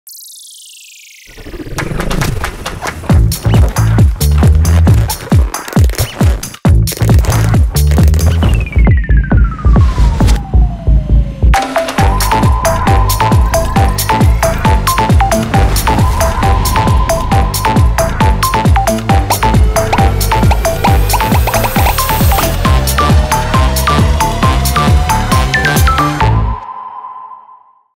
Samsung Privacy: Privacy Dashboard
Samsung Galaxy shows you at a glance which apps can access your data, and it helps you control them easily to keep your private data private. Your privacy. Secured.